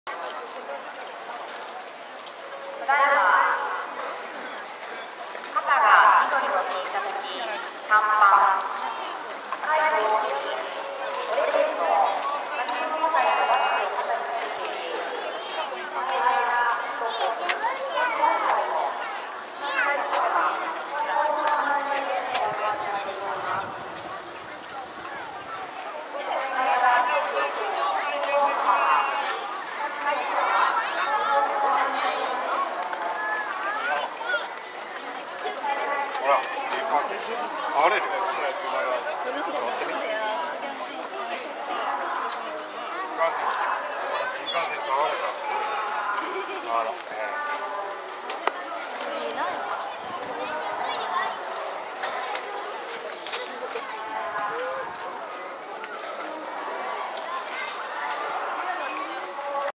100系 先頭車123-1
1985年に登場した2代目100系です。脳みそからの情報は1988年日立製造。東海所属になってからはX2編成として2002年まで活躍しました